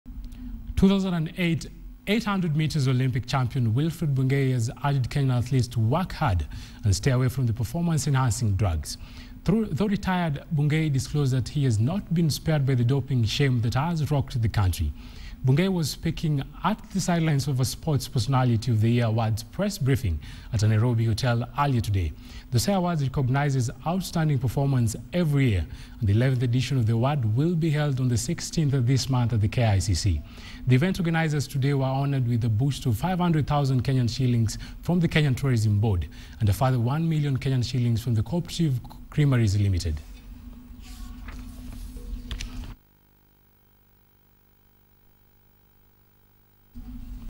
Wilfred Bungei urge Kenyan athletes to stay away from performance enhancing drugs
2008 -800m Olympics champion Wilfred Bungei has urged Kenyan athletes to work hard and stay away from performance enhancing drugs. Though retired, Bungei disclosed that he has not been spared the doping shame that has rocked the country.
Bungei was speaking at the sidelines of a sports personality of the year awards press briefing at a Nairobi hotel. The soya awards recognizes outstanding performances every year; and the 11th edition of the awards will be held on 16th of this month at KICC. The event organizers today were honored with a boost of 500,000 Kenya shillings from the Kenya tourism board and a further one million Kenya shillings from Kenya co-operative creameries ltd.